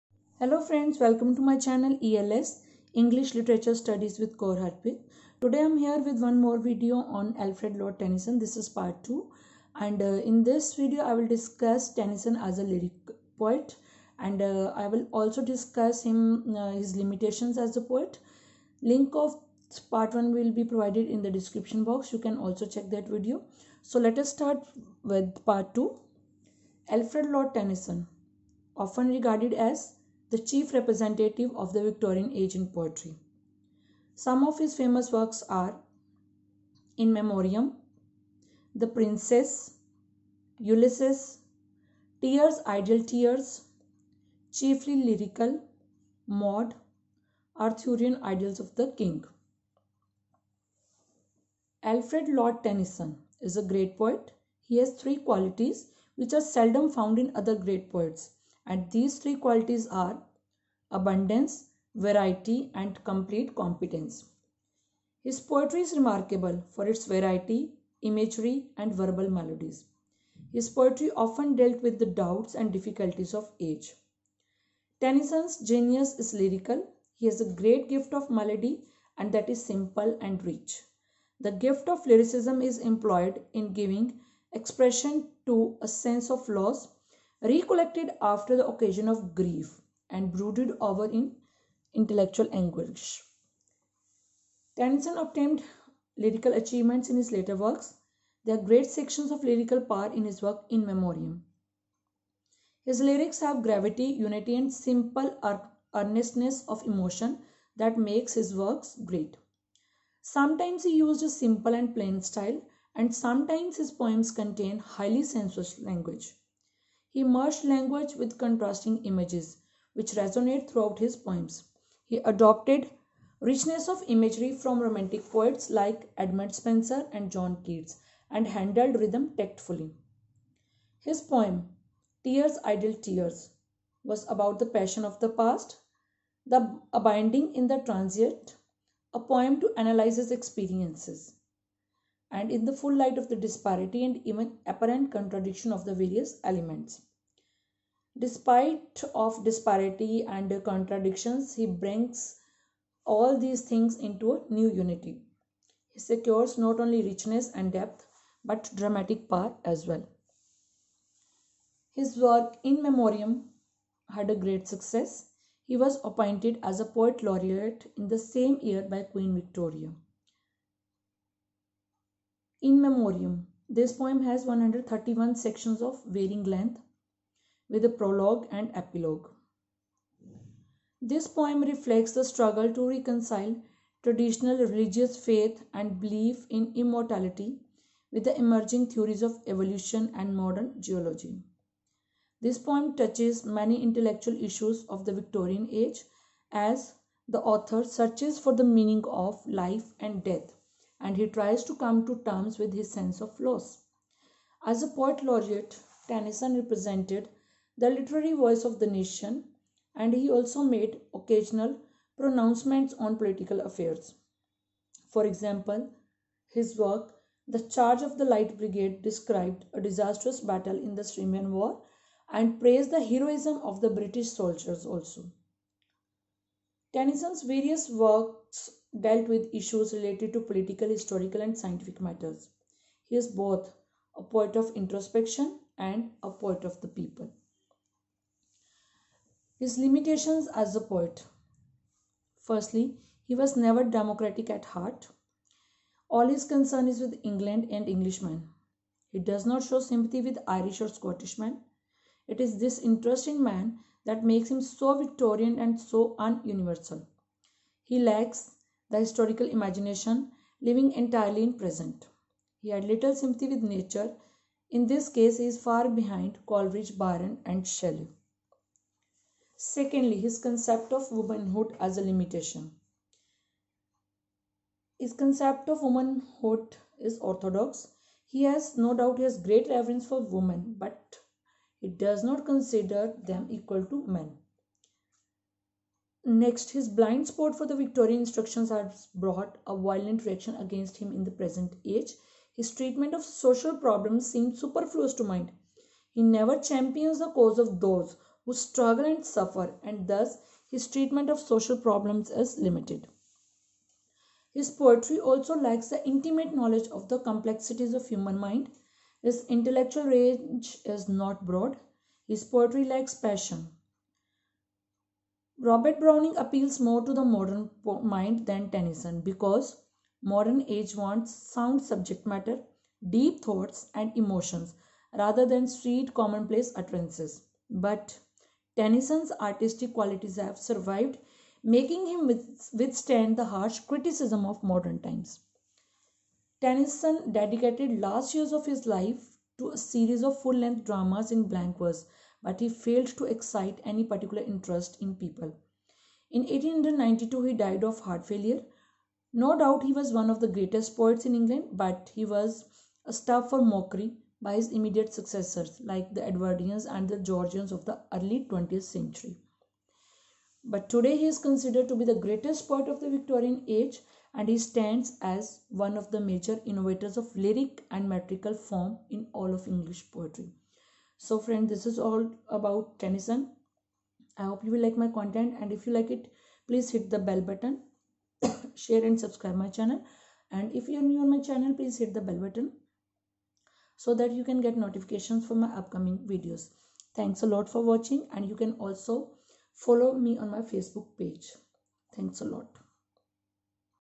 Alfred Lord Tennyson- Part II / Tennyson as a Lyric poet / Limitations of Tennyson as a poet
Hello Friends
Today’s video is about  Alfred Lord Tennyson  Part - II. In this video we will discuss him as a lyric poet and his limitations a sa poet